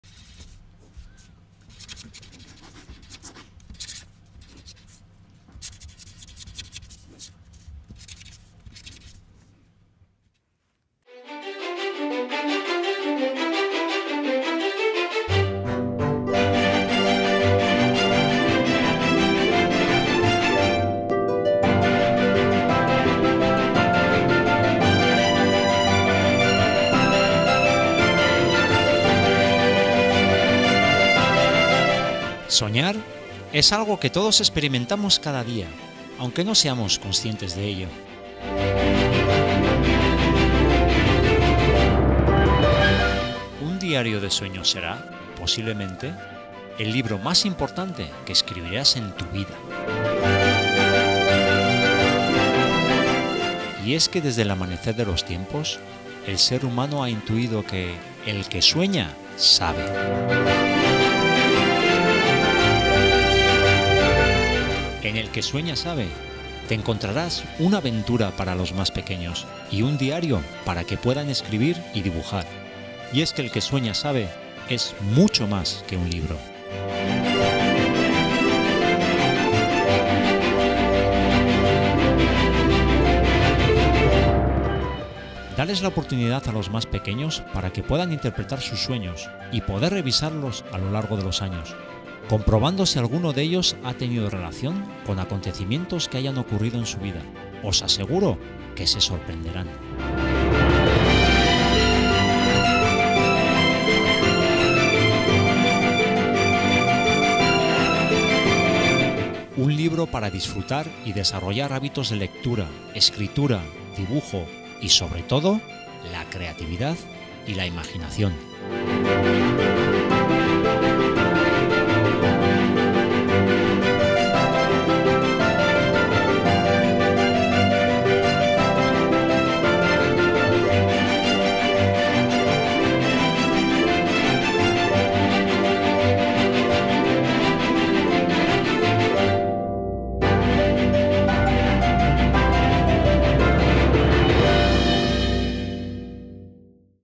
EL QUE SUEÑA, SABE... un diario de sueños
Iker Urdangarin Iraeta nos presenta una propuesta diferente y una aventura extraordinaria para los más pequeños: el libro EL QUE SUEÑA, SABE - Un diario de sueños. Un cuento infantil que despierta, según vamos descubriendo su relato, la curiosidad, el deseo de conocer y la propia autoestima de la persona y un diario de sueños donde poder recopilar los recuerdos, las sensaciones o las certezas experimentadas en las horas de sueño que tantas y tantas veces acompañamos de recuerdos y vivencias llevadas a cabo durante esos momentos mágicos que damos en llamar, sueños.

Puedes adquirir EL QUE SUEÑA, SABE en cualquier librería del país y a la propia editorial en esta dirección

A G A L I R  Ediciones